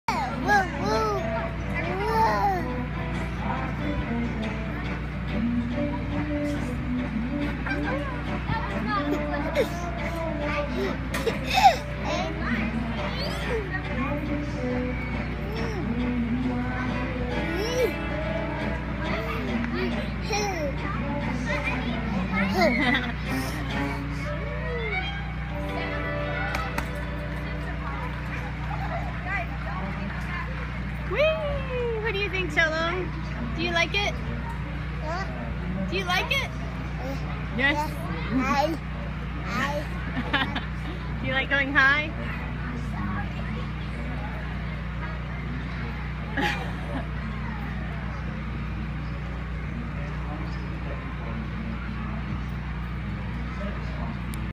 Bungee trampoline contraption
At the Bee Cave Galleria Halloween event